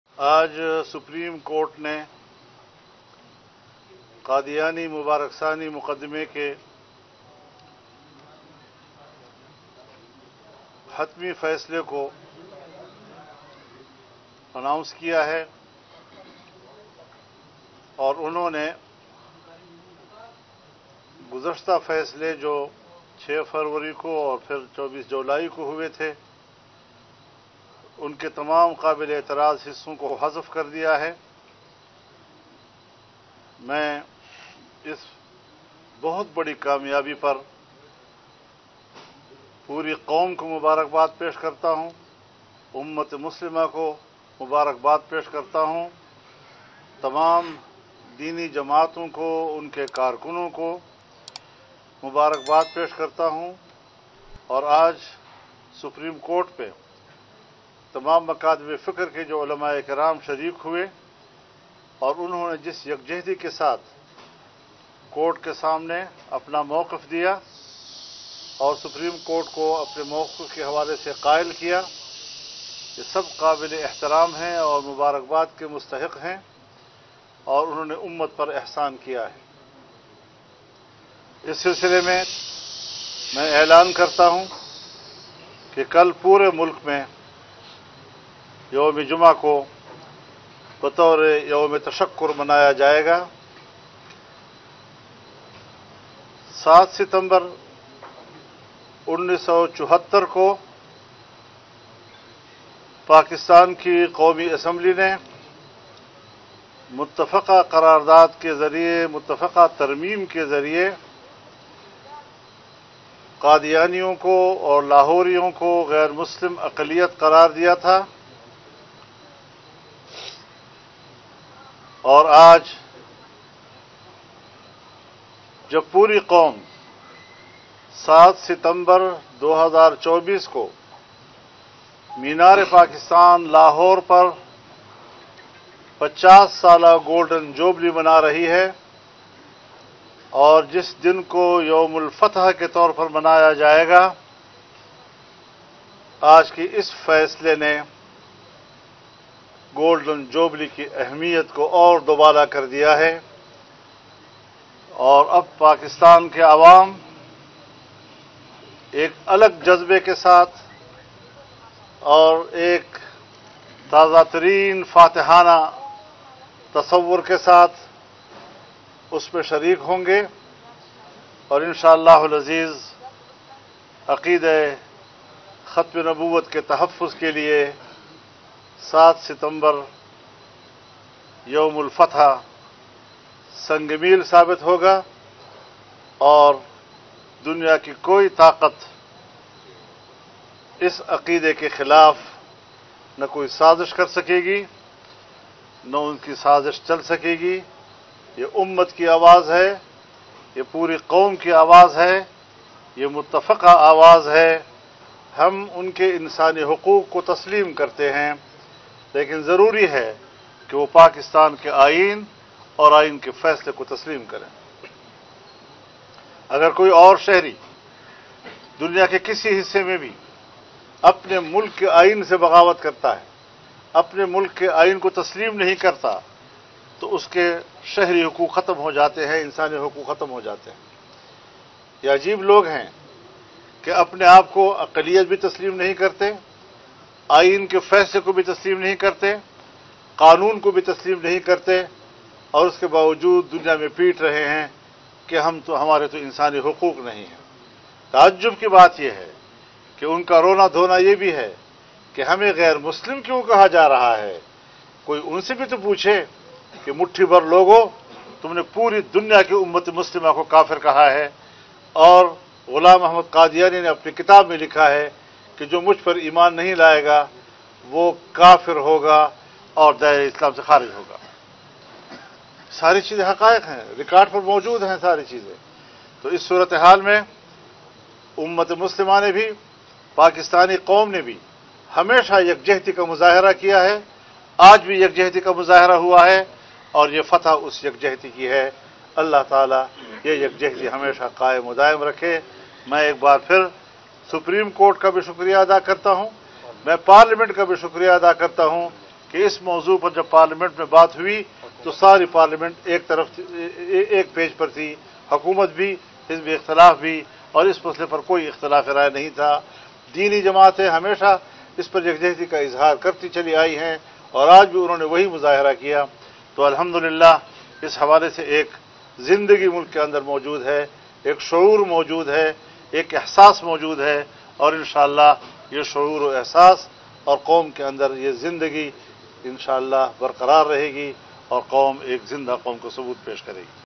Supreme Court Ne Kaunsa Bara Faisla Wapas Le Liya, Fazlur Rehman Ki Mubarakbad
Supreme Court Ne Kaunsa Bara Faisla Wapas Le Liya, Maulana Ki Ummat e Muslima Ko Mubarakbad

Supreme Court ne ek bara faisla wapas le liya hai jo aaj kal ke sab se zyada guftagu ka sabab ban gaya tha. Is video mein hum jaanenge ke Supreme Court ka kaunsa faisla wapas liya gaya aur is ka Maulana Fazlur Rehman aur Ummat-e-Muslima par kya asar hai. Dekhiye Maulana Fazlur Rehman ki taraf se is faislay par aayi mubarakbad aur is faislay ke political aur social implications ko samajhiye. Video ko dekhein aur update rahain